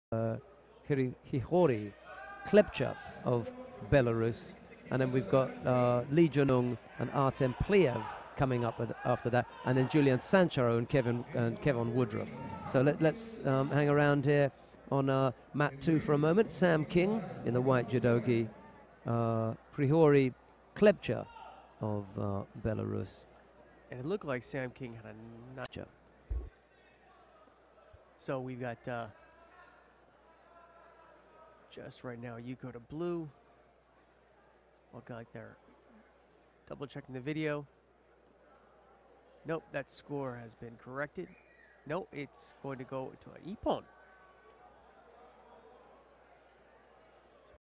Judo World Championships Cadets Miami 2013: King (AUS) - Klebcha (BLR) [-66kg]
Sam King (AUS) - Hryhory Klebcha (BLR) [-66kg]
Judo World Championships Cadets Miami 2013